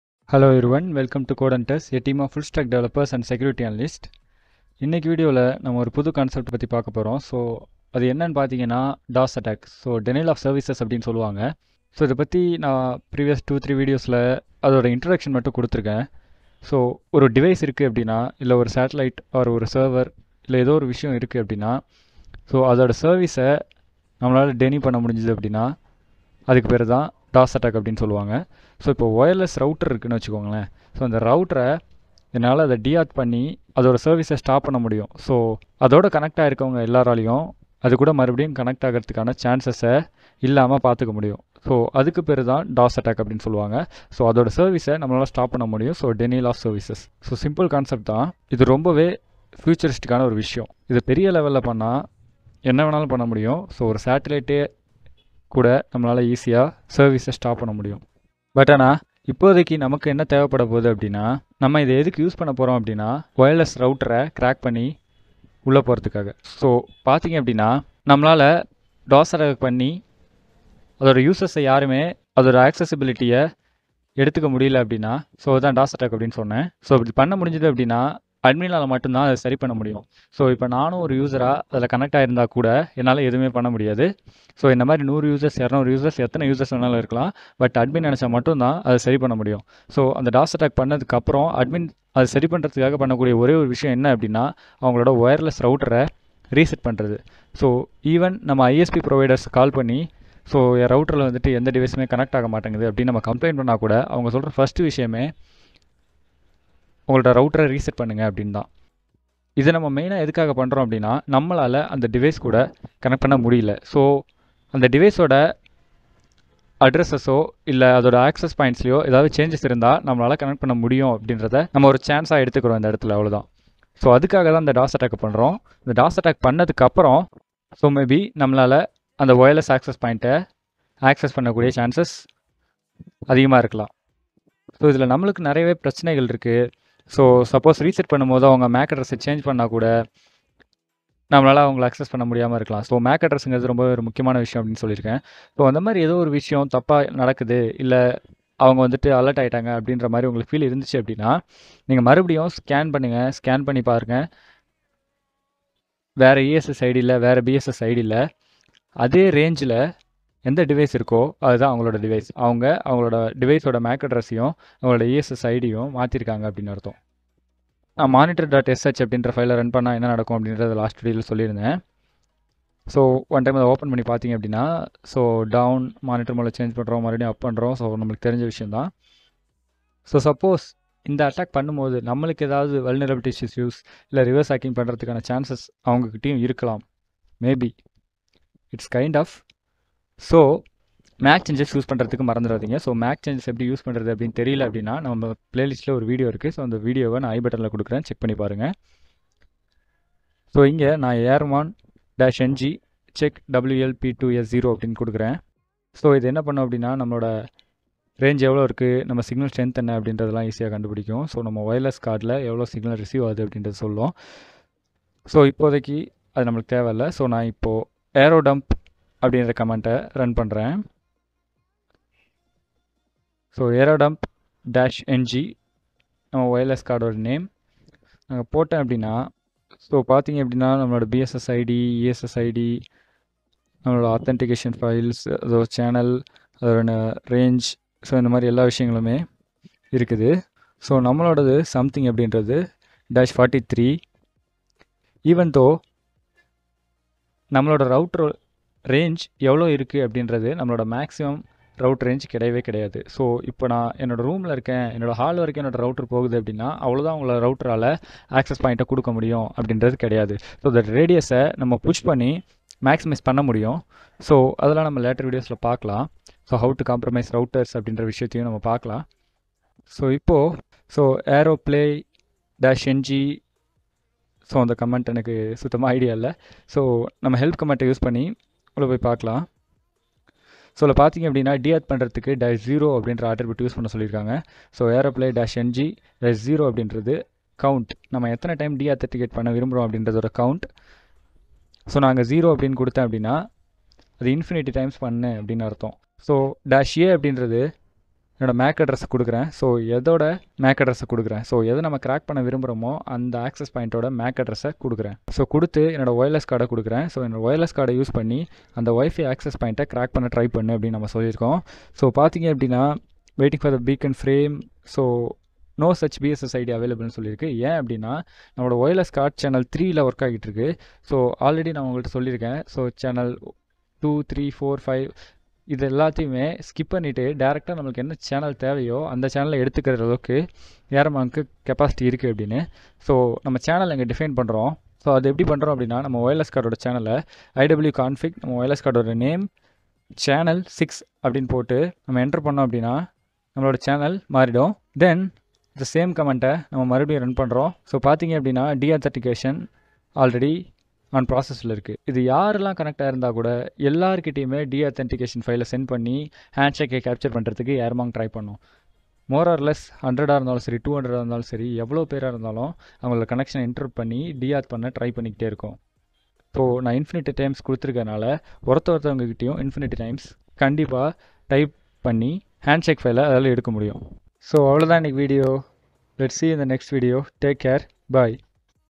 Dos - Attack Tamil | Kali Linux Tamil | Tamil Hacking | Ethical Hacking Tamil | Hack Wifi Dos Attack
What is Dos Attack ?

In computing, a denial-of-service attack is a cyber-attack in which the perpetrator seeks to make a machine or network resource unavailable to its intended users by temporarily or indefinitely disrupting services of a host connected to the Internet.